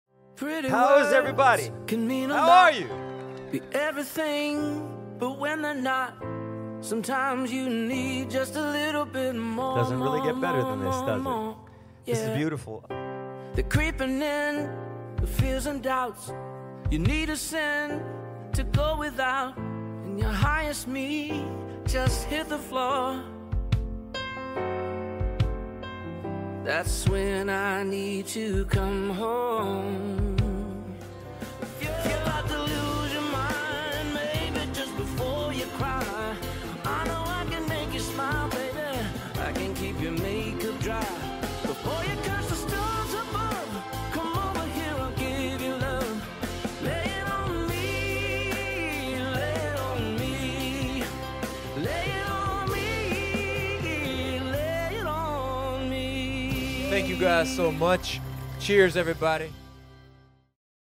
SIP NAPA 2023 Night 2 at Alpha Omega Winery
SIP NAPA 2023 ~ Night Two ~ @aowinery …
what a perfect night in Napa Valley!!! Gabe, Lauren, Charles, Pops, Slater, Wyatt, Gelbuda, Matraca & Jeff shared their talents with a beautiful crowd on an absolutely stunning wine country evening!  Thank you Alpha Omega… you guys are a SIP Napa OG winery and we love you for everything !!!

Performances from:
James Slater
Chris Gelbuda
Jeff Hanna